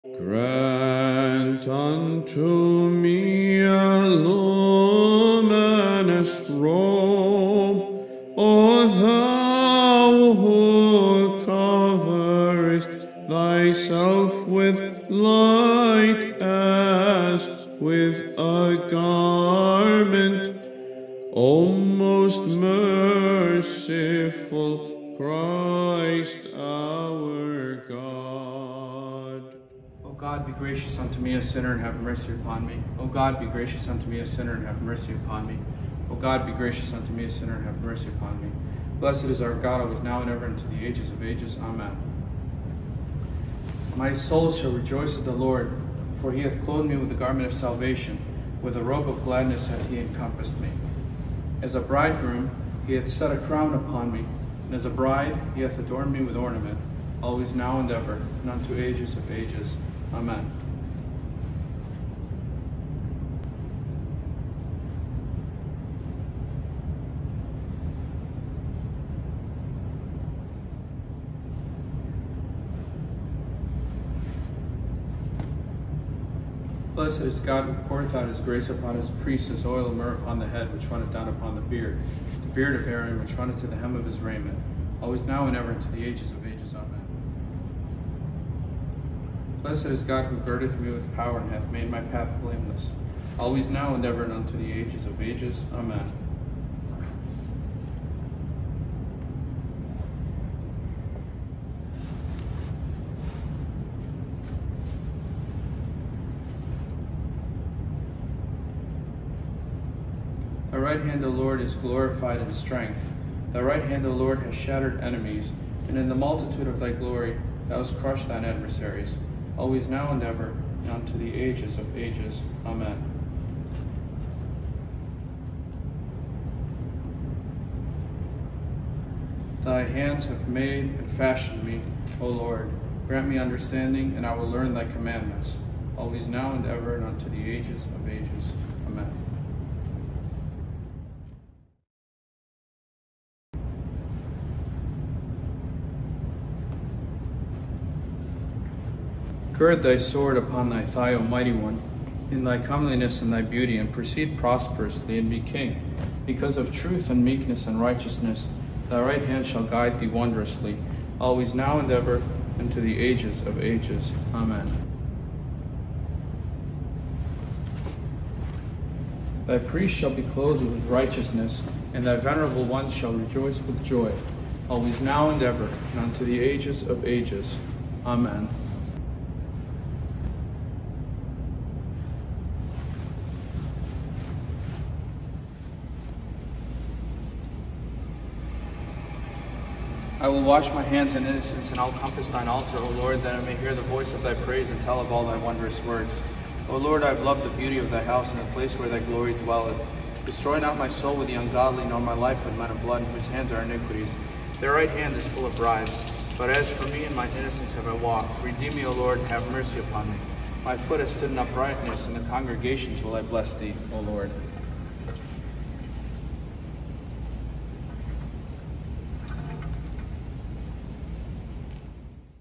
Vesting Service
Watch the second service of preparation where the priest puts on his vestments and washes his hands before the Divine Liturgy.